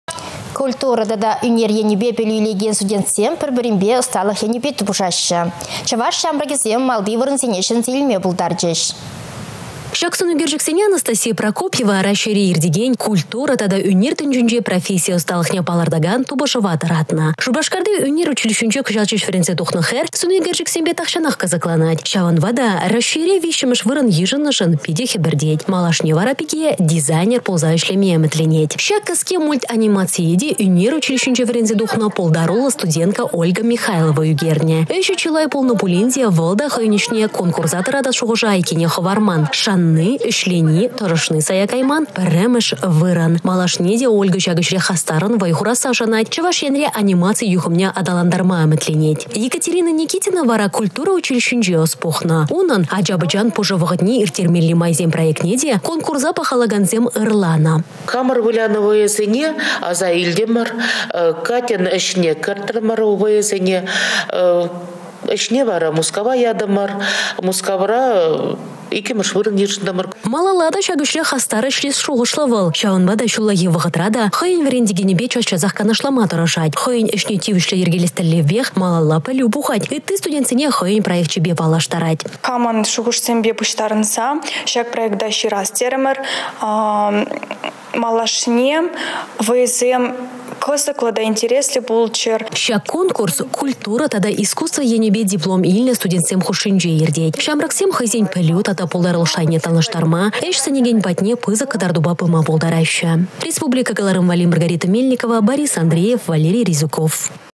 Чăваш Ен студенчĕсем малти вырăнсенче!
Культура тата ÿнер енĕпе пĕлу илекен студентсем пĕр-пĕринпе ăсталăх енĕпе тупăшаççĕ. Чăваш çамрăкĕсем малти вырăнсене çĕнсе илме пултарчĕç.